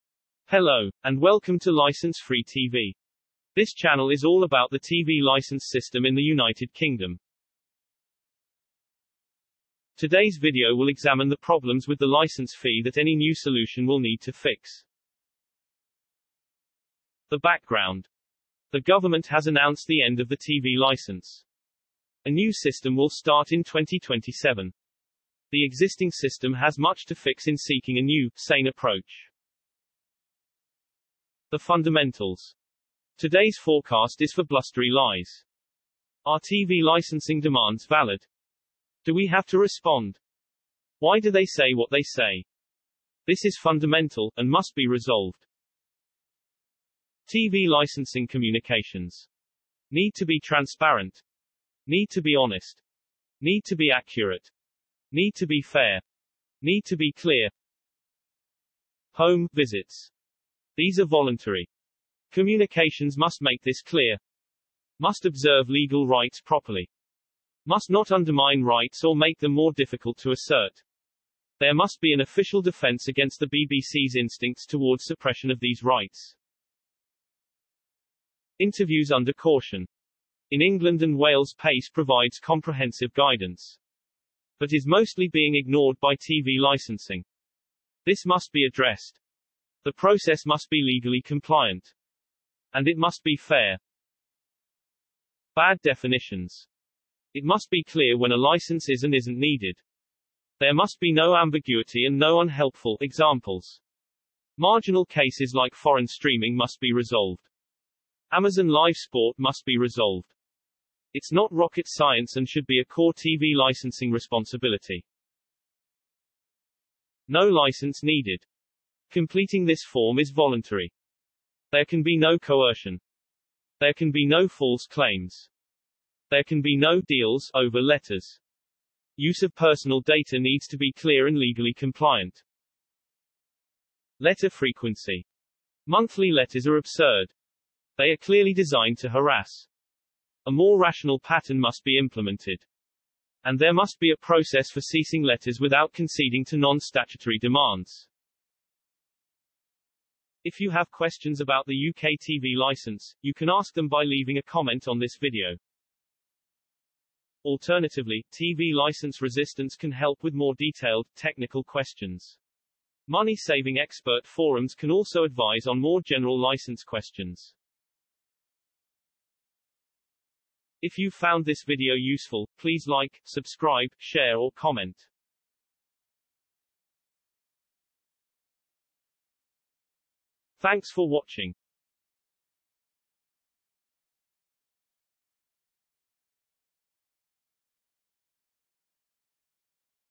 23 | TV Licensing: What needs to be fixed? | Licence Free TV
This video considers the elements of the existing TV Licensing regime that will need to be fixed in the forthcoming review.